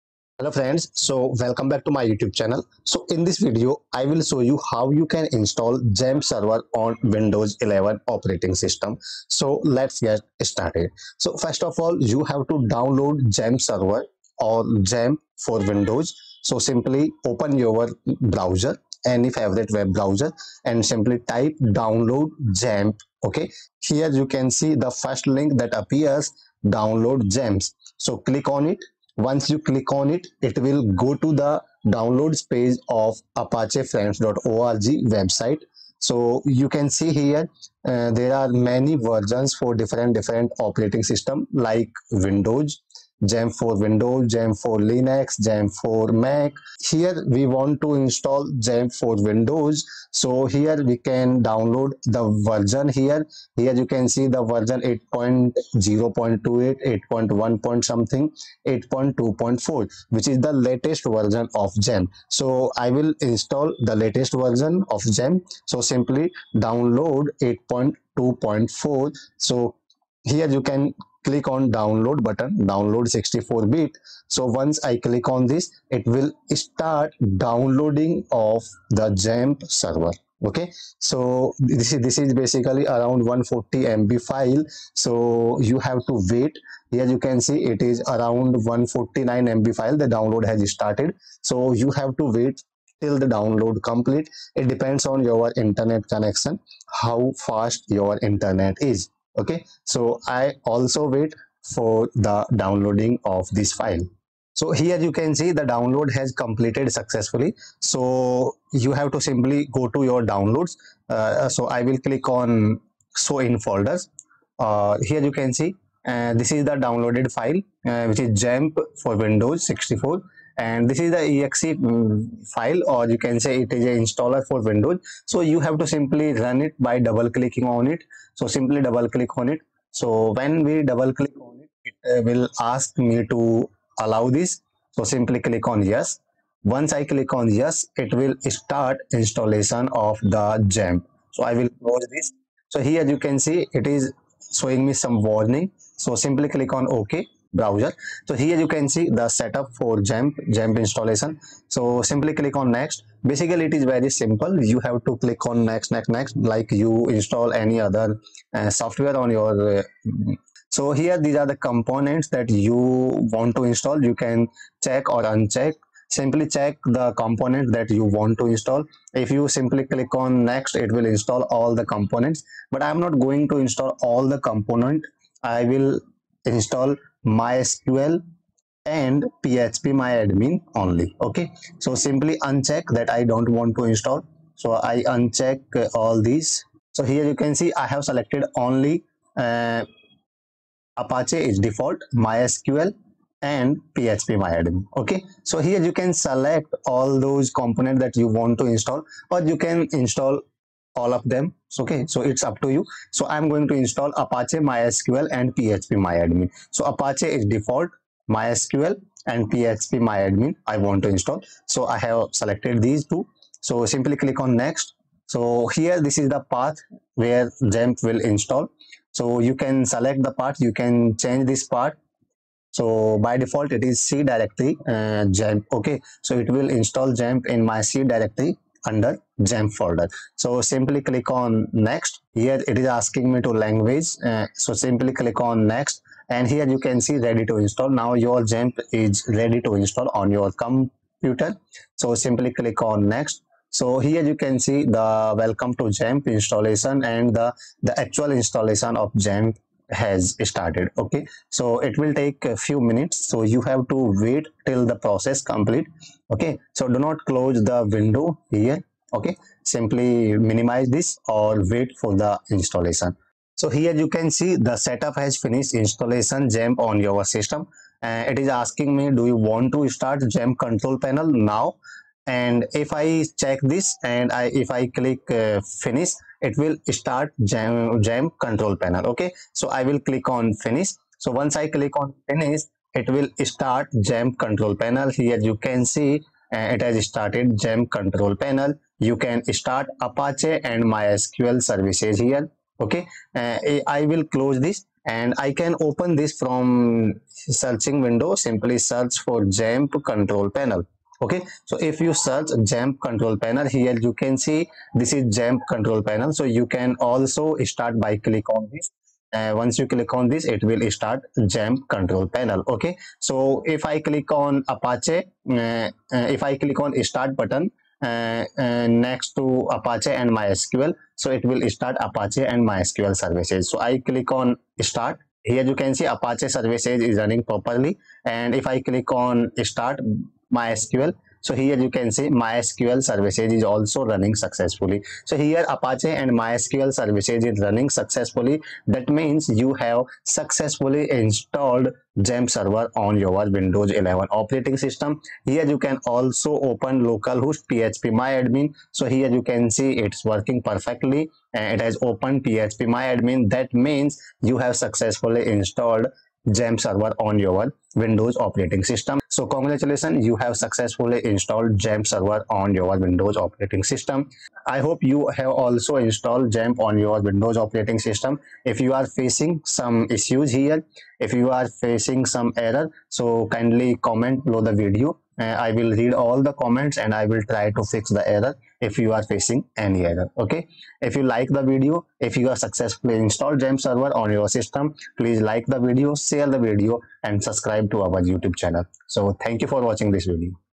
How to install XAMPP on Windows 11
In this video I have shown you How to install XAMPP server on windows 11.